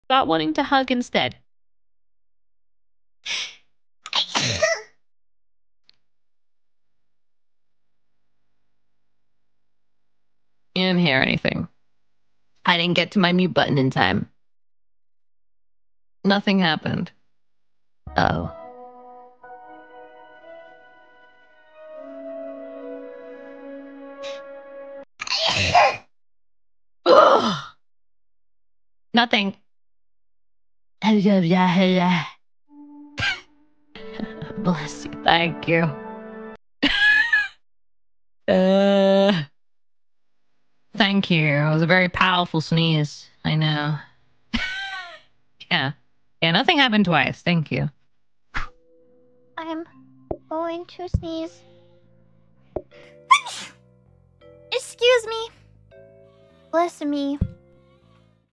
Elizabeth forgot to mute her sneezes and it's 𝐀𝐃𝐎𝐑𝐀𝐁𝐋𝐄 [Elizabeth Rose Bloodflame | HololiveEN]
Watch the original stream:
【RABBIT】Yey~! 💄